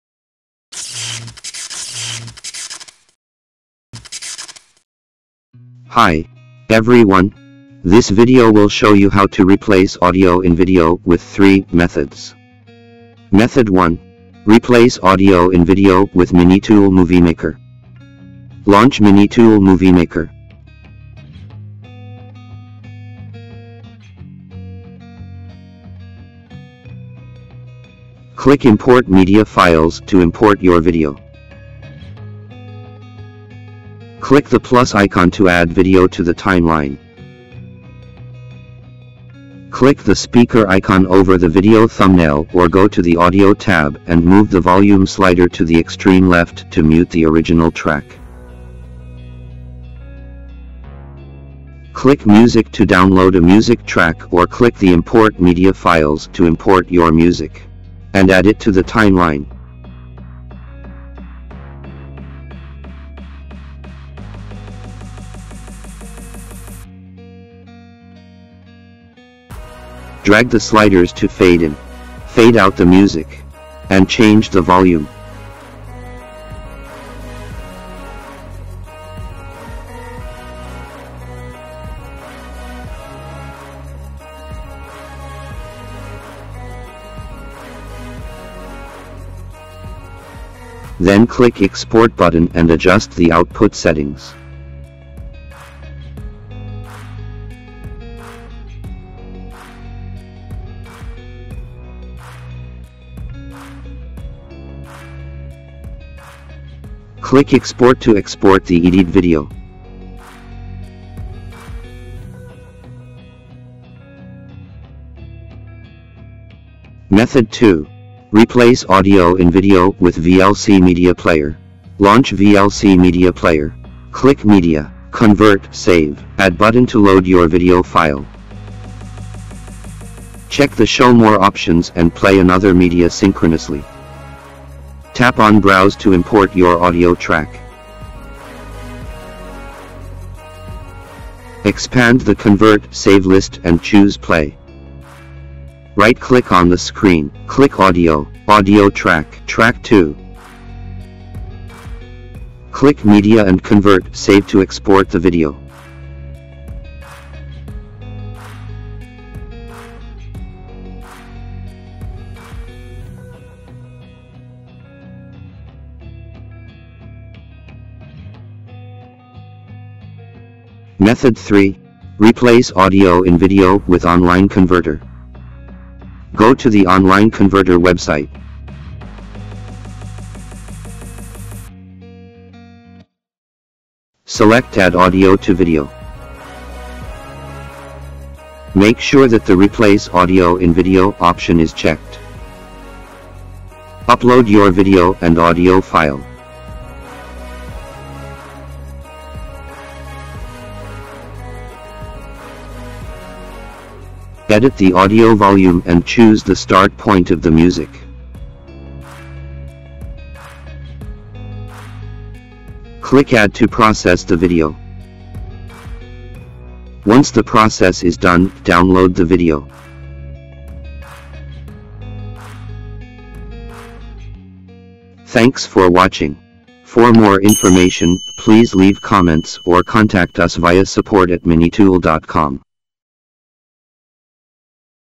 How to Replace Audio in Video Easily? 3 Methods
How to replace the audio in a video clip? This video offers 3 free ways to replace audio in the video.
Method 1. Replace audio in video with MiniTool MovieMaker 0:13 -1:51
Method 2. Replace Audio in Video with VLC Media Player  1:57-2:49
Method 3. Replace Audio in Video with Online Converter  2:51-3:43